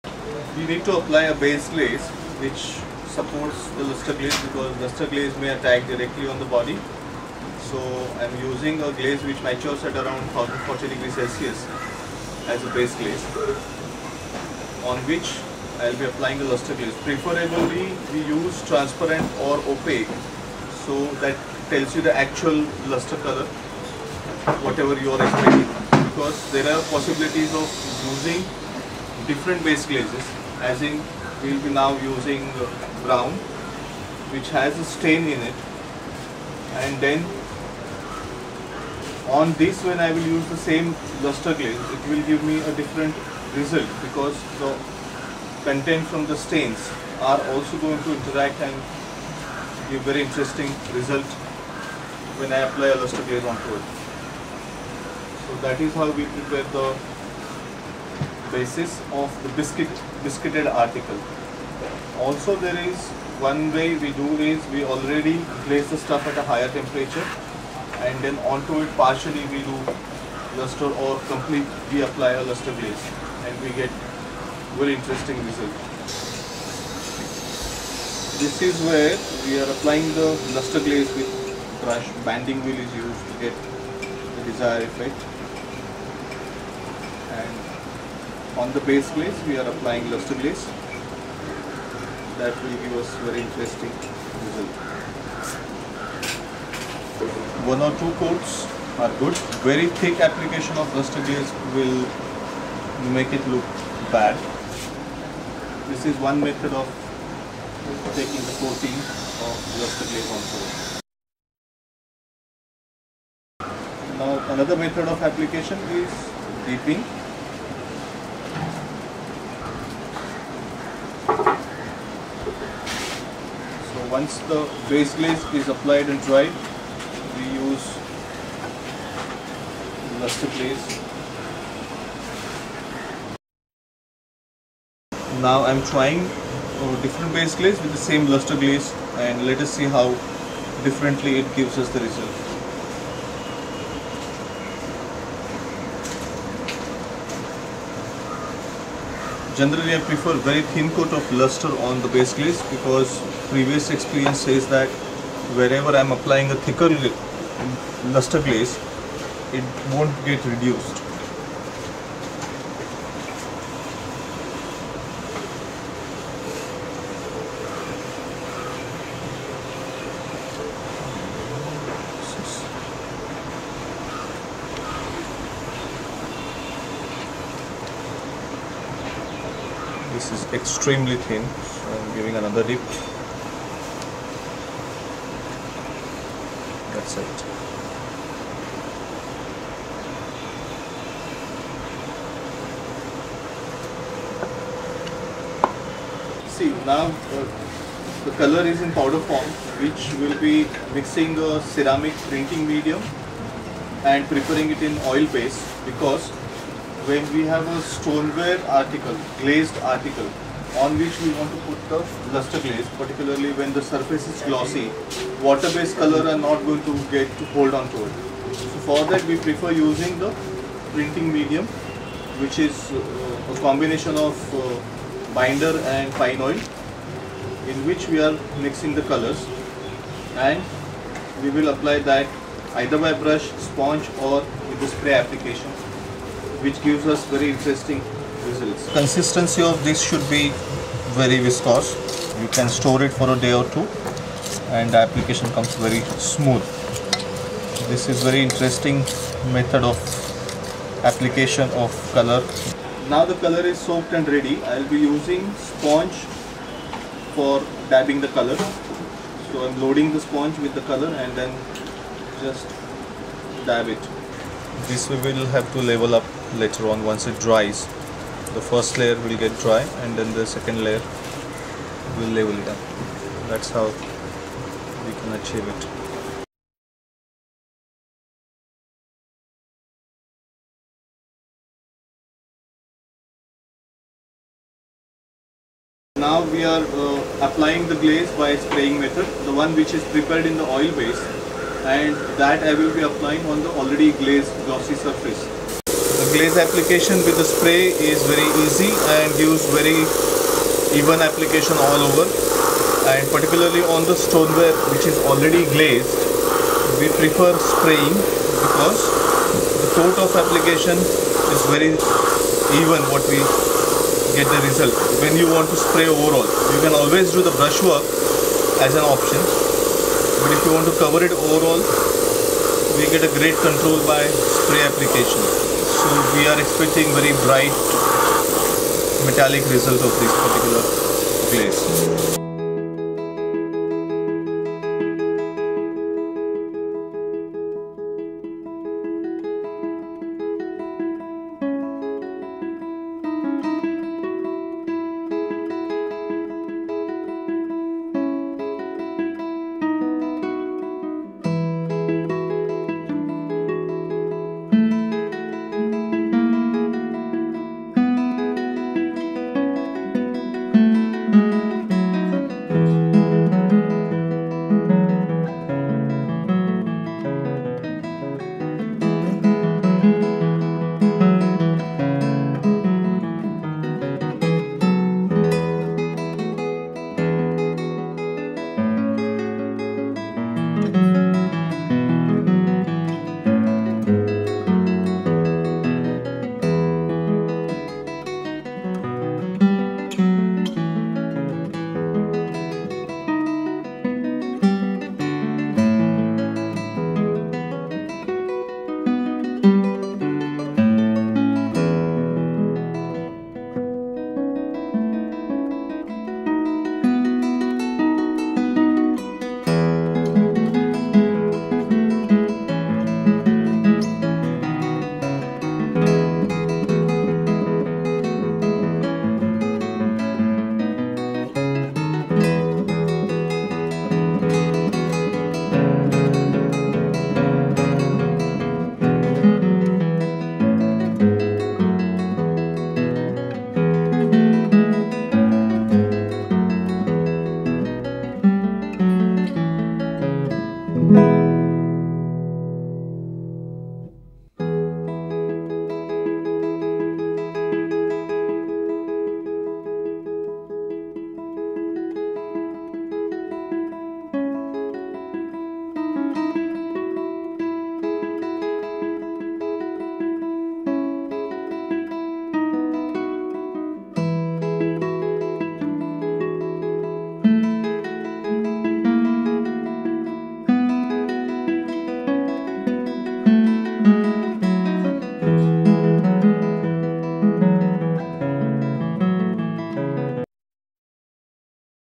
Ceramic Luster Glazing with Sandeep Manchekar
Sandeep is a ceramic artist and has in depth experience in the luster glazing technique.

Watch a demonstration of Luster Glazing by Sandeeo Manchekar.